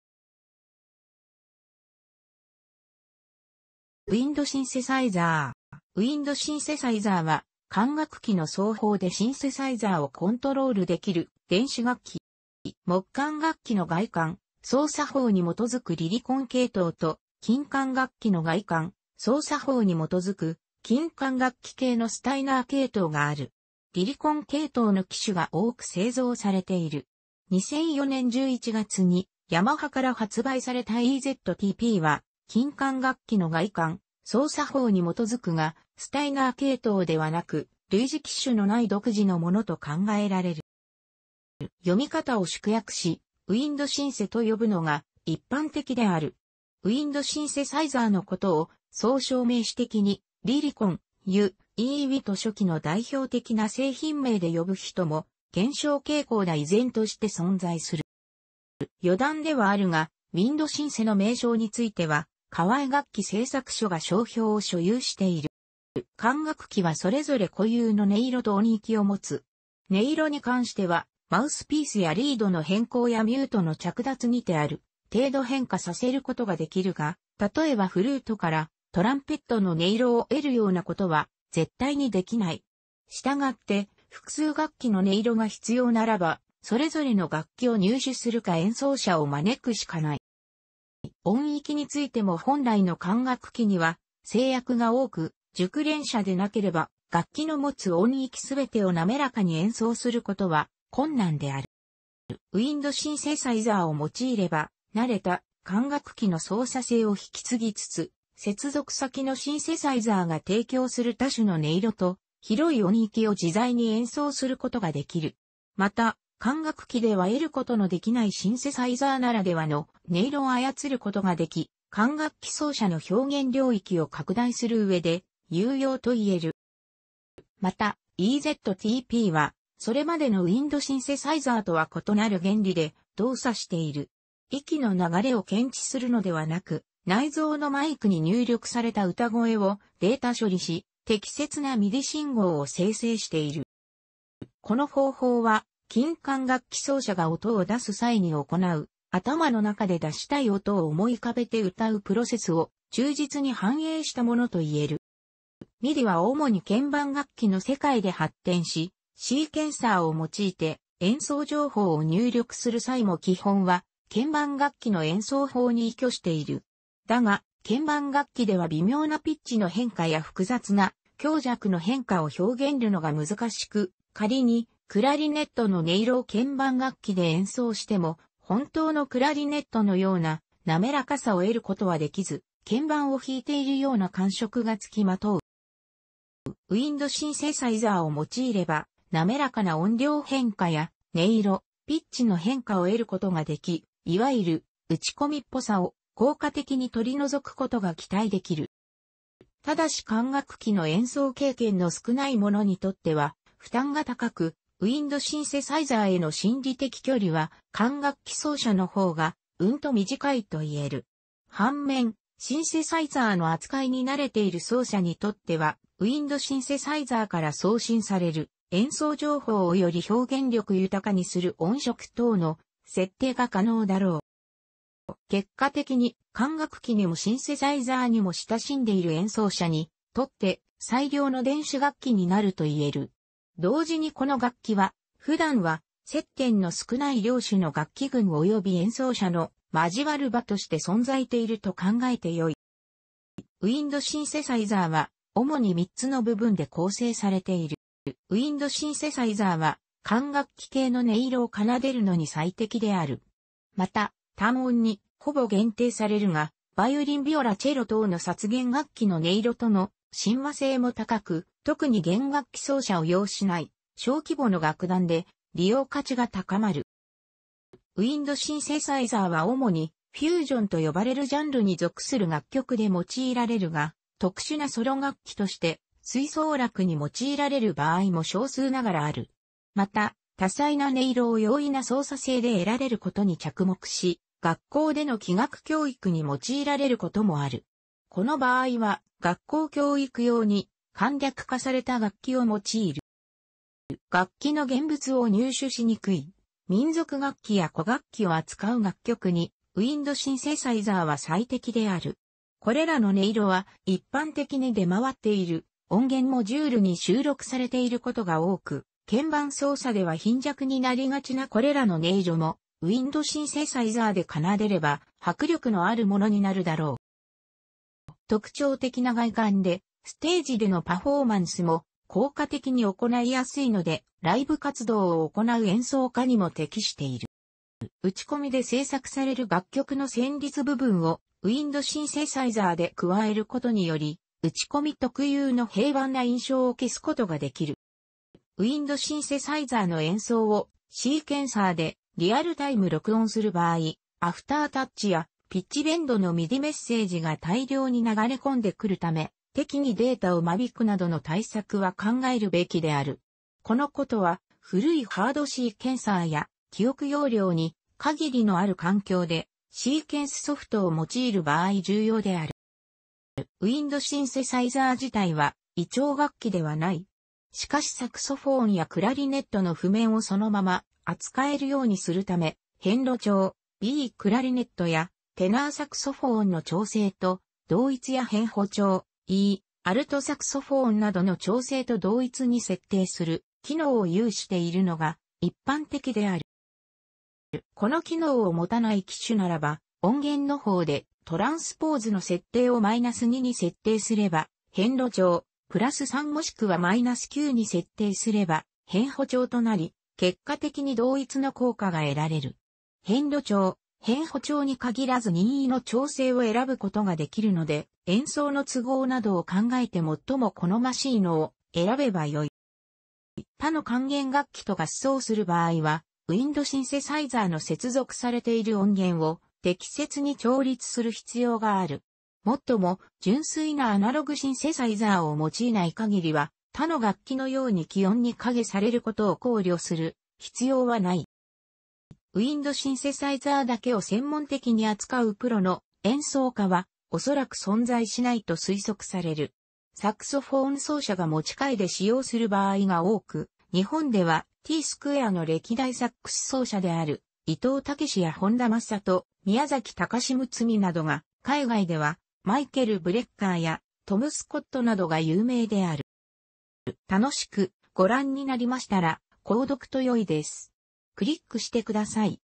ウインドシンセサイザー
ウインドシンセサイザー ("Wind Synthesizer") は、管楽器の奏法でシンセサイザーをコントロールできる電子楽器。

木管楽器の外観・操作法に基づくリリコン系統と金管楽器の外観・操作法に基づく金管楽器系のスタイナー系統がある。リリコン系統の機種が多く製造されている。2004年11月にヤマハから発売されたEZ-TPは金管楽器の外観・操作法に基づくがスタイナー系統ではなく、類似機種のない独自のものと考えられる。

読み方を縮約し「ウインドシンセ」と呼ぶのが一般的である。ウインドシンセサイザーのことを総称名詞的に「リリコン」、「EWI」（イーウィ）と初期の代表的な製品名で呼ぶ人も減少傾向だが依然として存在する。

余談ではあるが、「ウィンドシンセ」の名称については、河合楽器製作所が商標を所有している。(第2716074号)

管楽器はそれぞれ固有の音色と音域を持つ。
音色に関してはマウスピースやリードの変更やミュートの着脱にてある程度変化させることができるが、たとえばフルートからトランペットの音色を得るようなことは絶対にできない。従って複数楽器の音色が必要ならば、それぞれの楽器を入手するか演奏者を招くしかない。

音域についても本来の管楽器には制約が多く、熟練者でなければ楽器の持つ音域すべてを滑らかに演奏することは困難である。

ウインドシンセサイザーを用いれば、慣れた管楽器の操作性を引き継ぎつつ、接続先のシンセサイザーが提供する多種の音色と広い音域を自在に演奏することができる。また、管楽器では得ることのできないシンセサイザーならではの音色を操ることができ、管楽器奏者の表現領域を拡大する上で有用といえる。

また、EZ-TPは、それまでのウインドシンセサイザーとは異なる原理で動作している。息の流れを検知するのではなく、内蔵のマイクに入力された歌声をデータ処理し、適切なMIDI信号を生成している。

この方法は金管楽器奏者が音を出す際に行う「頭の中で出したい音を思い浮かべて歌う」プロセスを忠実に反映したものといえる。

MIDIは主に鍵盤楽器の世界で発展し、シーケンサーを用いて演奏情報を入力する際も基本は鍵盤楽器の演奏法に依拠している。だが、鍵盤楽器では微妙なピッチの変化や複雑な強弱の変化を表現するのが難しく、仮にクラリネットの音色を鍵盤楽器で演奏しても、本当のクラリネットのような滑らかさを得ることはできず、鍵盤を弾いているような感触がつきまとう。

ウインドシンセサイザーを用いれば、滑らかな音量変化や音色・ピッチの変化を得ることができ、いわゆる「打ち込みっぽさ」を効果的に取り除くことが期待できる。

ただし管楽器の演奏経験の少ない者にとっては負担が高く、ウインドシンセサイザーへの心理的距離は管楽器奏者の方がうんと短いといえる。反面、シンセサイザーの扱いに慣れている奏者にとっては、ウインドシンセサイザーから送信される演奏情報をより表現力豊かにする音色等の設定が可能だろう。

結果的に、管楽器にもシンセサイザーにも親しんでいる演奏者にとって最良の電子楽器になるといえる。同時にこの楽器は、普段は接点の少ない両種の楽器群および演奏者の交わる場として存在していると考えてよい。

ウインドシンセサイザーは主に三つの部分で構成されている。

ウインドシンセサイザーは管楽器系の音色を奏でるのに最適である。また、単音にほぼ限定されるが、バイオリン・ビオラ・チェロ等の擦弦楽器の音色との親和性も高く、特に弦楽器奏者を擁しない小規模の楽団で利用価値が高まる。

ウインドシンセサイザーは主にフュージョンと呼ばれるジャンルに属する楽曲で用いられるが、特殊なソロ楽器として吹奏楽に用いられる場合も少数ながらある。また、多彩な音色を容易な操作性で得られることに着目し、学校での器楽教育に用いられることもある。この場合は学校教育用に簡略化された楽器を用いる。

楽器の現物を入手しにくい民族楽器や古楽器を扱う楽曲にウインドシンセサイザーは最適である。これらの音色は一般的に出回っている音源モジュールに収録されていることが多く、鍵盤操作では貧弱になりがちなこれらの音色も、ウインドシンセサイザーで奏でれば迫力のあるものになるだろう。

特徴的な外観で、ステージでのパフォーマンスも効果的に行いやすいので、ライブ活動を行う演奏家にも適している。

打ち込みで制作される楽曲の旋律部分をウインドシンセサイザーで加えることにより、打ち込み特...